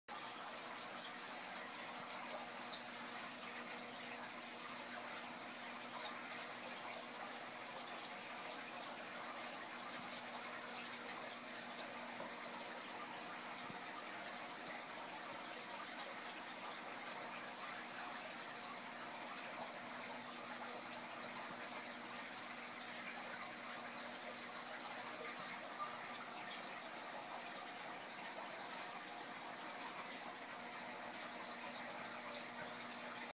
Hungry earth eaters
Geos and guianacara begging for food